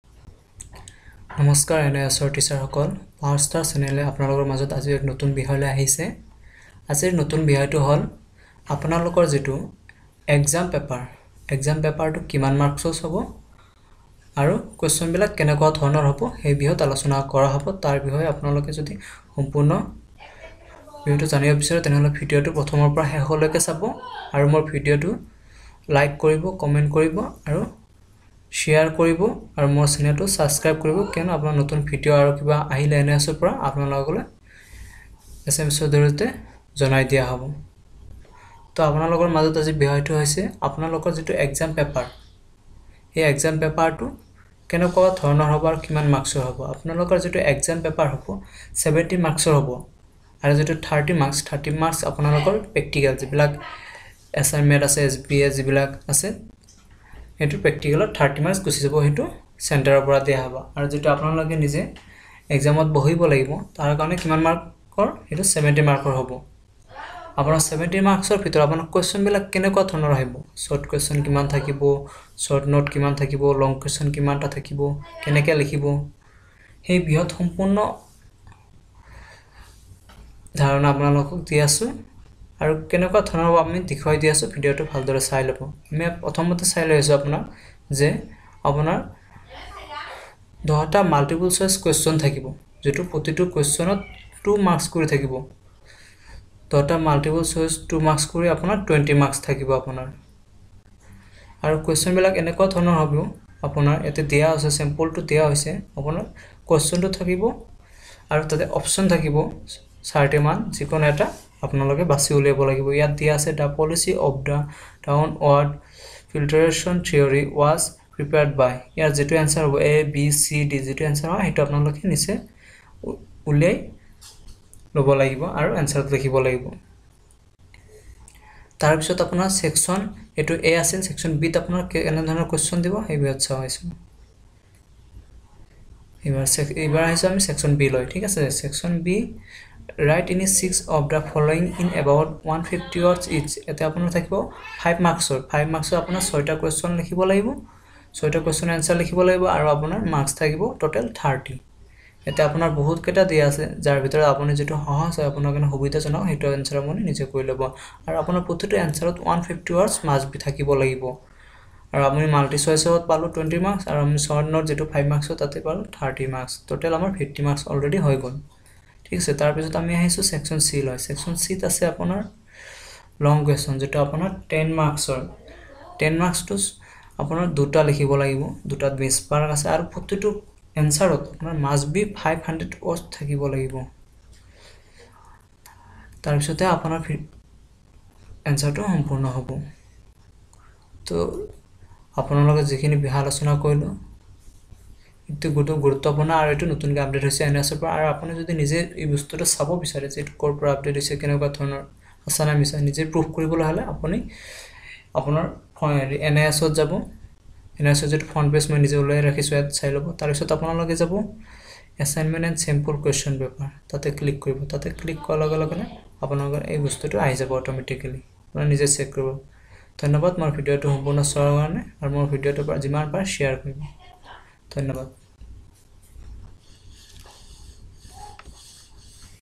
NIOS deled Question Paper Sample in assamese language for Sem-1 block 501, 502 & 503
Max. Marks:70 Duration: 3 Hrs
Section –A
There are 10 multiple choice questions in this section. All are compulsory
Multiple Choice Questions :— 2x10 = 20
1. The policy of the ‘Down ward filtration theory’ was prepared by
 A. Wood’s despatch
 B. Lord Macaulay
 C. Hunder Commission
 D. Sadler Commission
2. The Government of India established National Council of Educational Research and
Training (NCERT) in the year—
 A. 1951
 B. 1961
 C. 1971
 D. 1981
3. Which article of the constitution includes 10 fundamental duties of citizens?
 A. Article 45
 B. Article 21
 C. Article 51A
 D. Article 21A
4. Elementary Education in India is —
 A. From class I to class X
 B. From class I to class V
 C. From class I to class VIII
 D. From class V to class VIII
5. Universalisation of Elementary Education with free and compulsory education was
proposed in
 A. NEP 1986 and the plan of Action 1992
 B. SSA and Knowledge Commission
 C. Knowledge Commission and the Plan of Action 1992
 D. Kothari Commission and NEP1986
6. Bridge School model was adopted for—
 A. Special Children
 B. Rural Children
 C. Urban Children
 D. Out of the School Children
7. Full name of the DPEP is
 A. Direct Programme for Education Planning
 B. District Primary Education Programme
 C. Distance Programme for Education and Planning
 D. District Planning for Education Programme
8. Participants of Jomitien Conference re affirmed the right of:
 A. Gender Equality
 B. Education of Adult
 C. Every Citizen
 D. Education HIV/AIDS affected children
9. E-9 counties means—
 A. Electronic countries
 B. Economical advance countries
 C. High population countries
 D. Economical backward countries
10. Right to free and compulsory Education for all children in the age group 6-14 years has
given the Article—
 A. 24 of the Indian Constitution
 B. 15 of the Indian Constitution
 C. 26 of the Indian Constitution
 D. 21A of the Indian Constitution
Section-B
Write any six of the following in about 150 words each. 6x5 = 30
 11. Write down the objectives of SSA
 12. State the reasons for non-achievement of goals of UEE
 13. State the major thrust areas for shaping the visions of Jomtien Conference.
 14. What are the steps for involving community in micro planning process.
 15. State the rights of the child as declared by the United Nations.
 16. State the roles and responsibilities of BRCs and CRCs in developing elementary
education.
 17. Why is reflective teaching important in elementary education.
 18. Why is inclusive education important at elementary education level.
 19. Write down the salient features of operation blackboard of UEE as stated in
NPE 1986.
 20. State the main features of basic education as given in Wardha scheme of 1937.
Section – C
Write any two of the followings in about 500 words each : 2
                        10=20
 21. Analyse the state of education of minority children in your state.
 22. Write down the impact of DPEP an elementary education in your state.
 23. Which of the quality of the ancient Guru you would like to practice as a
professional teacher — Give three reasons and explain them.
 24. What are the advantages of inclusive educatio